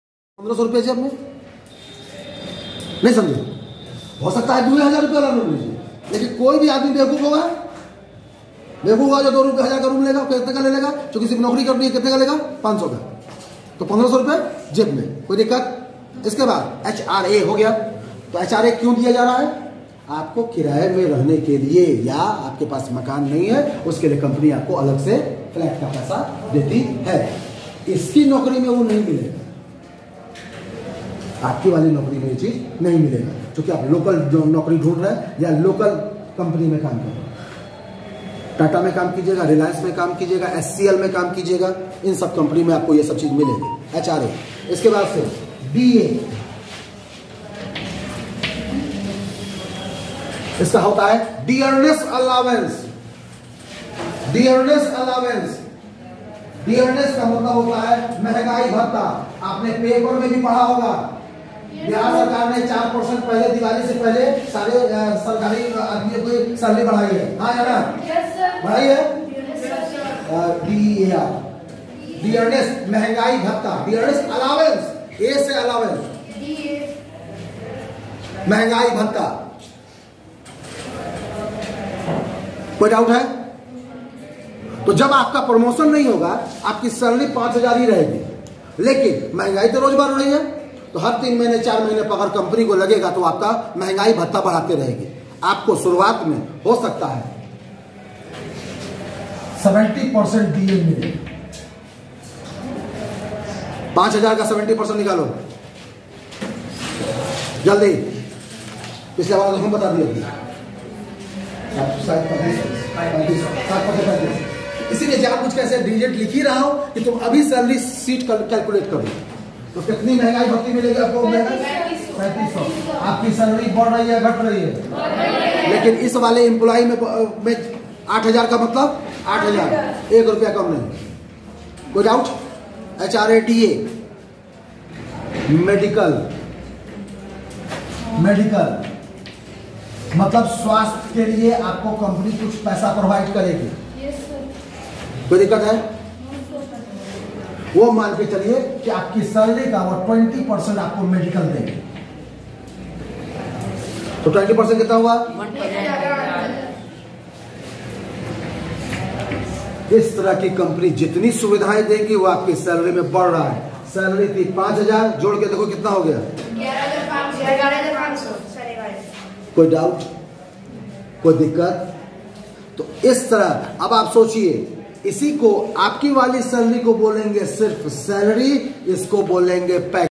7A - 5th of 16th Parts - How to create a normal Salary Sheet in MS EXCEL
What is Employee?
What is Employer?
What is Salary Sheet?
What is plain or normal Salary?
What is Package Salary?
What is Criteria?
How to create a normal Salary Sheet in MS EXCEL?
What is HRA, DA, TA, MEDICAL etc.
How we can calculate Net Pay